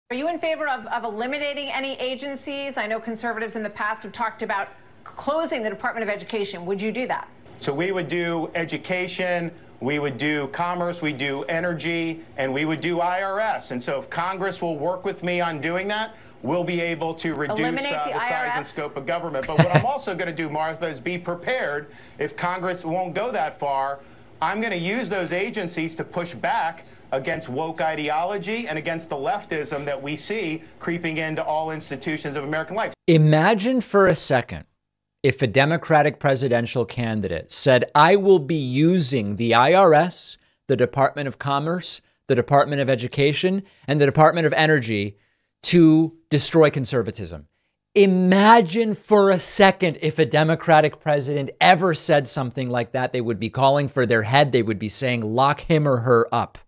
Ron DeSantis' plan is so insane, even Fox host doesn't buy it
-- 2024 Republican presidential candidate Ron DeSantis claims he will eliminate the IRS, Department of Education, Commerce, and Energy in an interview so absurd, even Fox News host Martha MacCallum is visibly skeptical
Leave a Voicemail Line: (219)-2DAVIDP

Broadcast on June 29, 2023